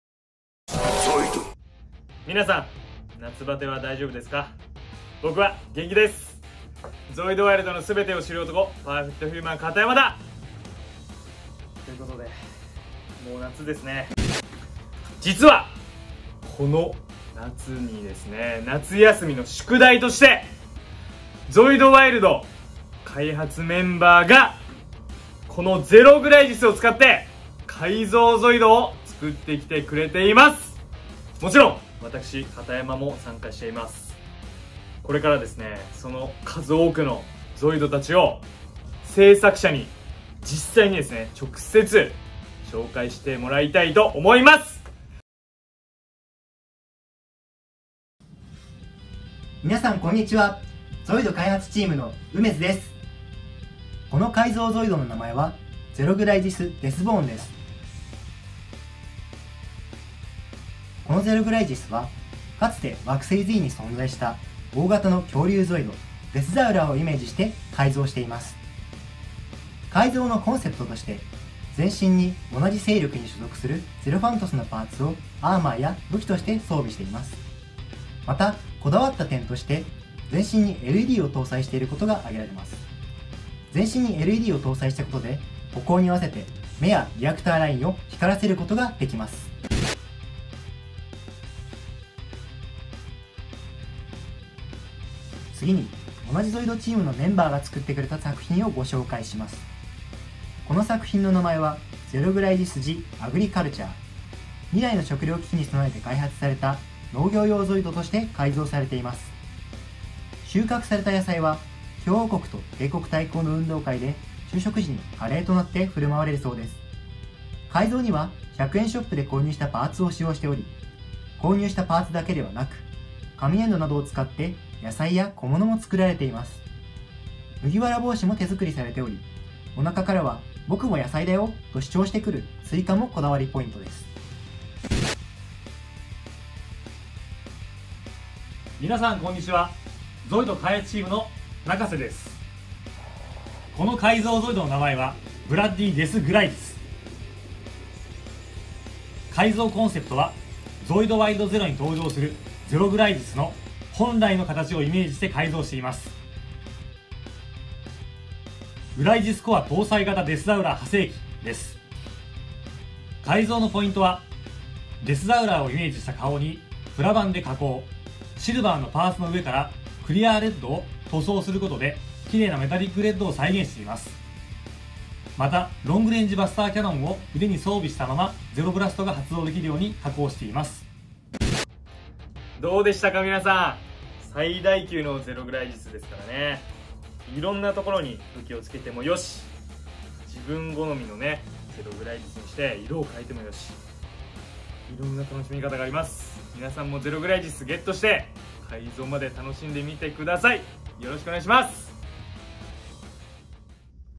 【ゾイドワイルド】第一弾！夏休みの宿題 改造ゼログライジスを紹介！
. ゾイド開発チームに課された夏休みの宿題！
それは…
【改造ゼログライジス】を製作せよ！

日々、新たなゾイドを生み出すことに力戦奮闘するゾイド開発チーム。

そのゾイド開発チームによる、改造ゼログライジスを製作者自ら紹介するぞ！
今回はその第一弾！
君もゼログライジスを改造してみよう！